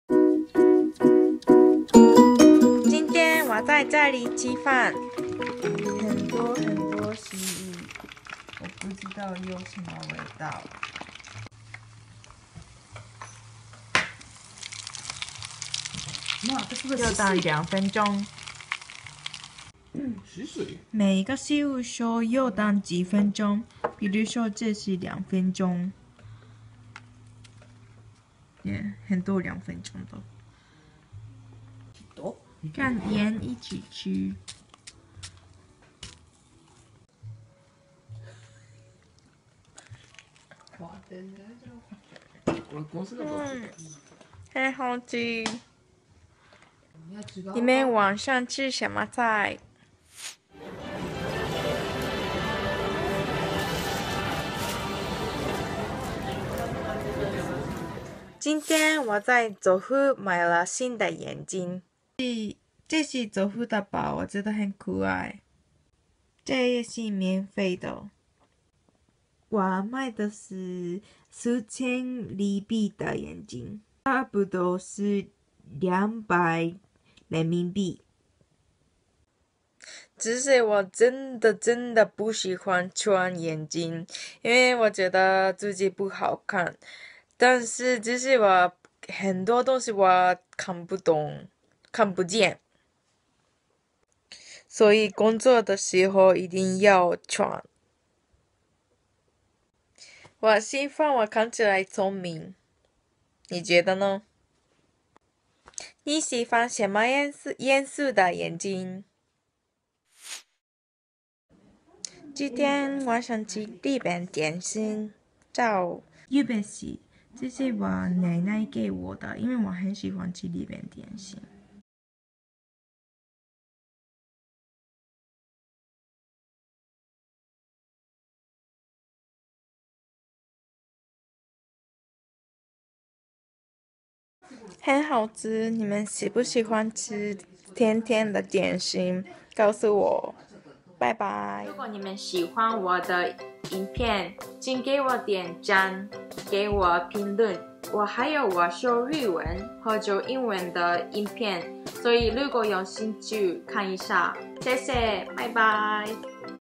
【日本人說中文】Japanese girl's Vlog日本人が中国語でビデオブログに挑戦！
日本女生說中文日記。日本女生说中文日记。
日本人が中国語でビデオブログに挑戦！
Japanese gir's Vlog in Chinese. My life in Japan!